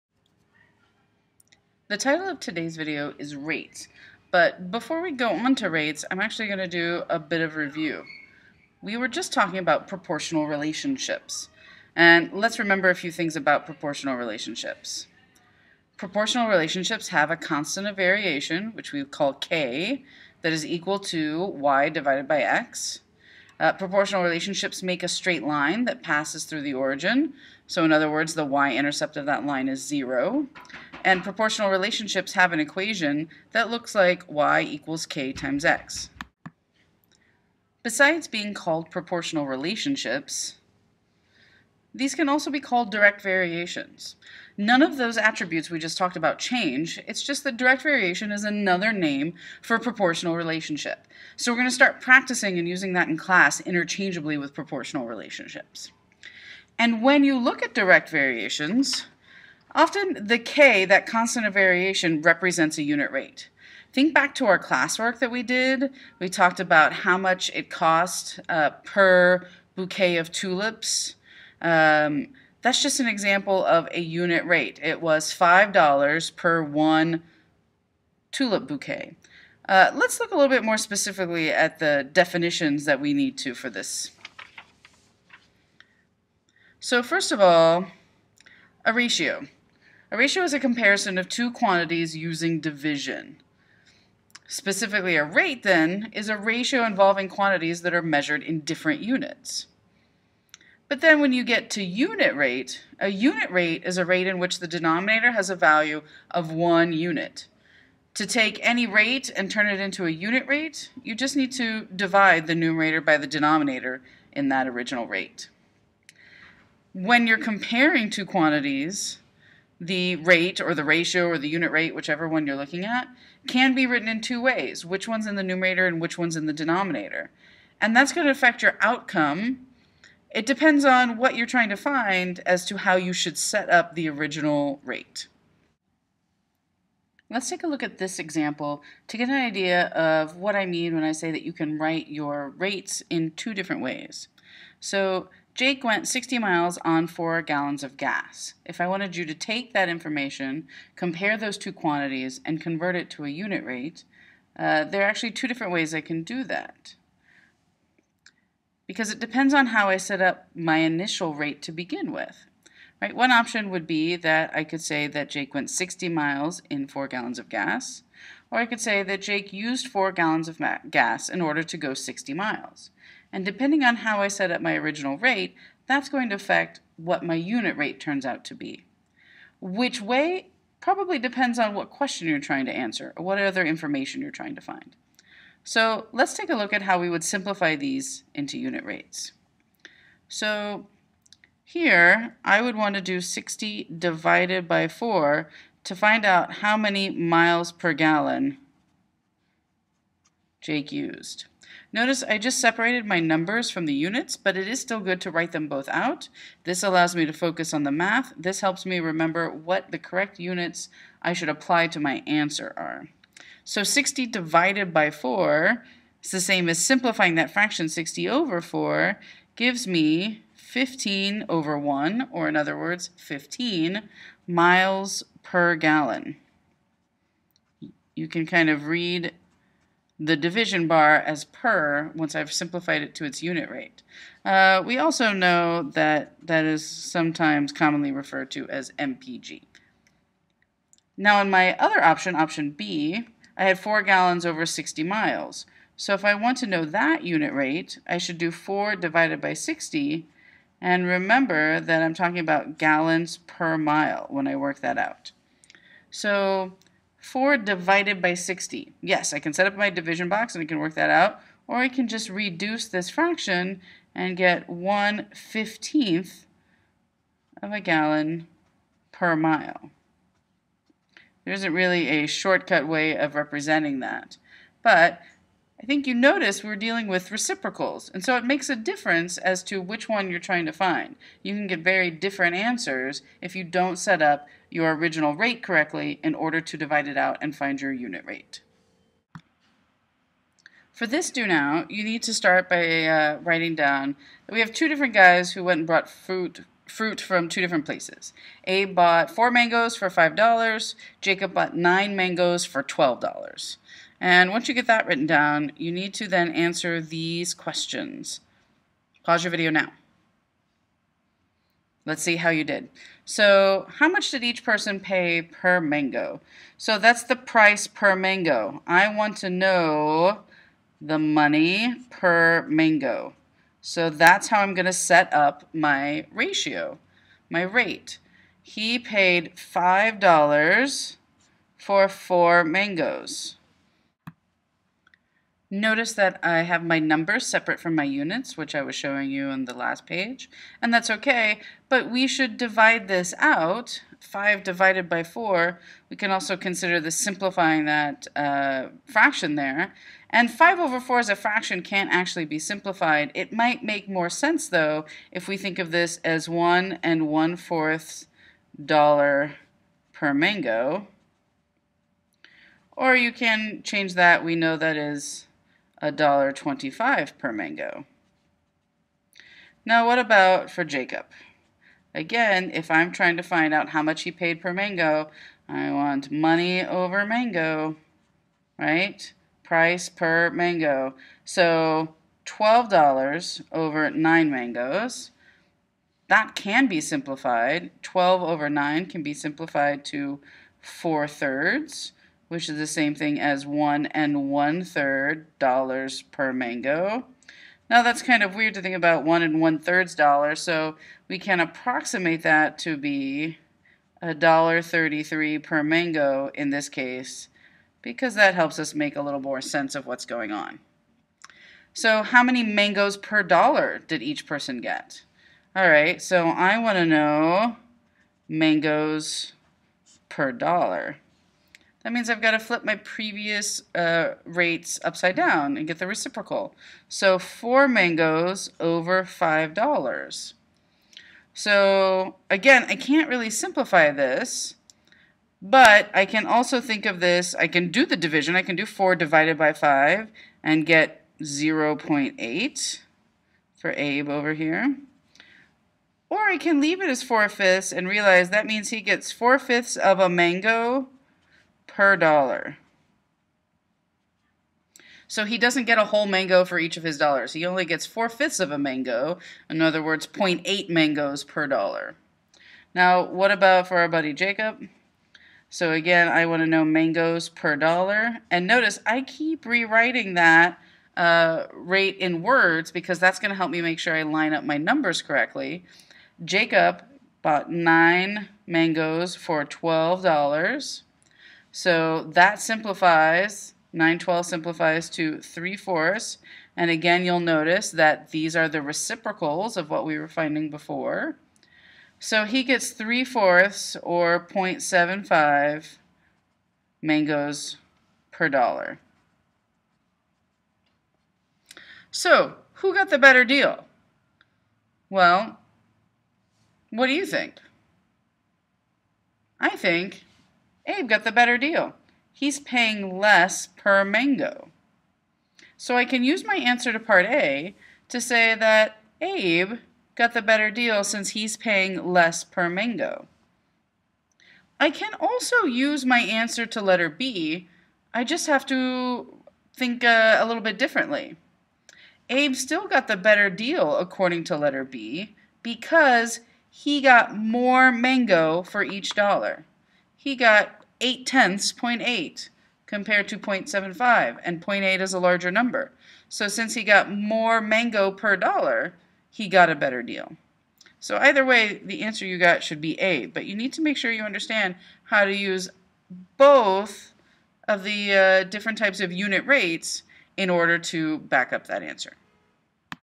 Rates
See how our work with proportional relationships (a.k.a. direct variations!) leads into a discussion of ratios, rates, and unit rates.